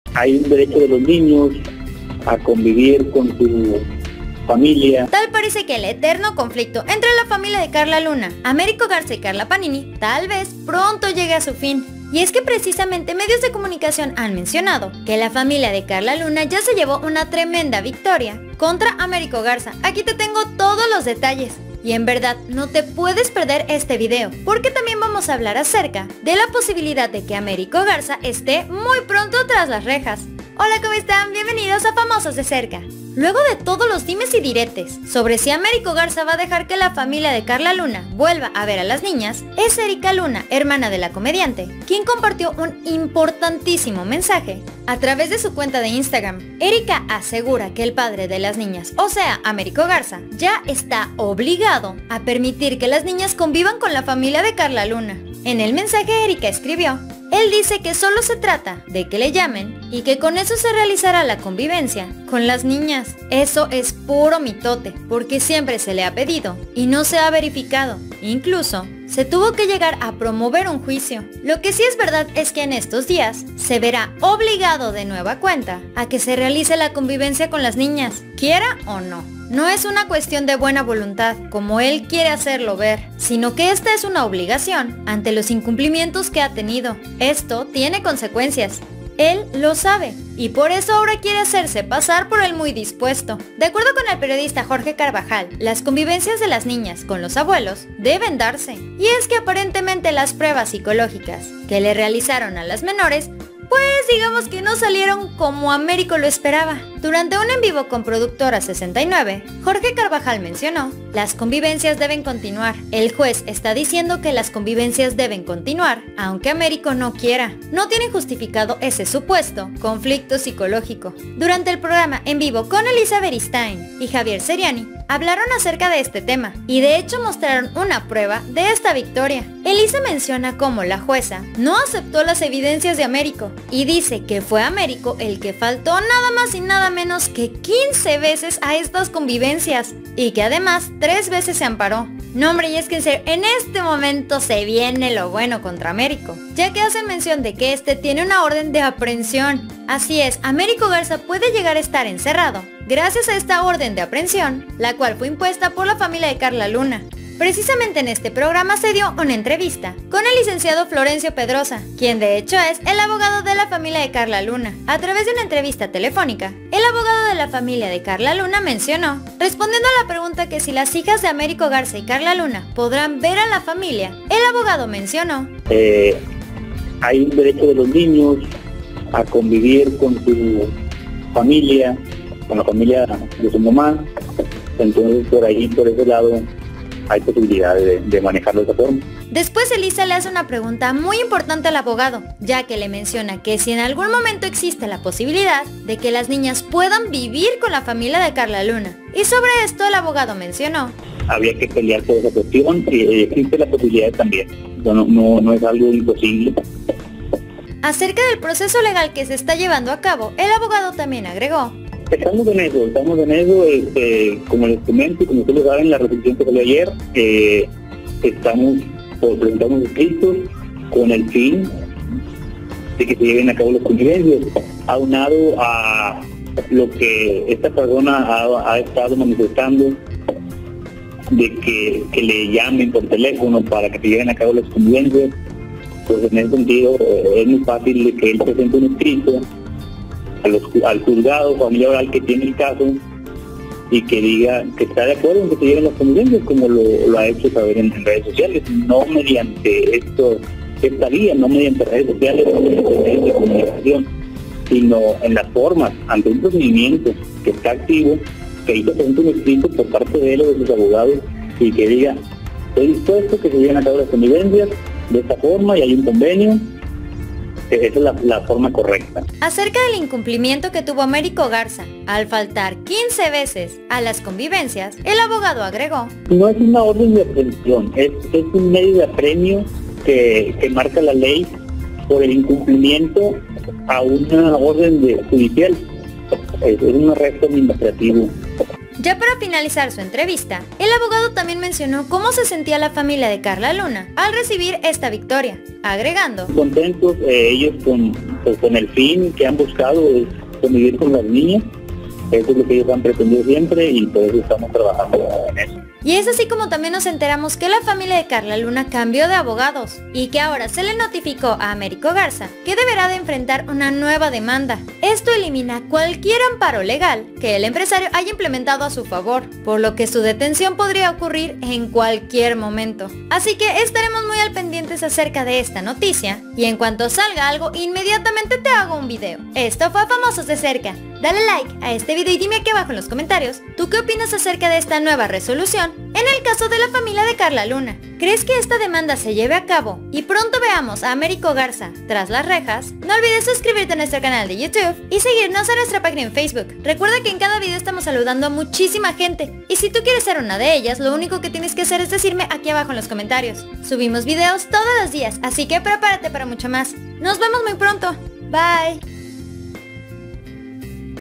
⛔😱 ¡ 𝗗𝘂𝗿𝗼 R𝗲𝘃é𝘀 para Americo Garza 🚨 𝗙𝗮𝗺𝗶𝗹𝗶𝗮 𝗱𝗲 𝗞𝗮𝗿𝗹𝗮 𝗟𝘂𝗻𝗮 𝗢𝗯𝘁𝗶𝗲𝗻𝗲 𝗩𝗶𝗰𝘁𝗼𝗿𝗶𝗮 ! 🔴💥
Sin duda es 𝘂𝗻𝗮 𝗴𝗿𝗮𝗻 𝗮𝗹𝗲𝗴𝗿𝗶𝗮 𝗽𝗮𝗿𝗮 𝗹𝗮 𝗳𝗮𝗺𝗶𝗹𝗶𝗮 𝗹𝘂𝗻𝗮 despues 𝗱𝗲 𝘁𝗮𝗻𝘁𝗼 𝘁𝗶𝗲𝗺𝗽𝗼.

 BIENVENIDOS!!!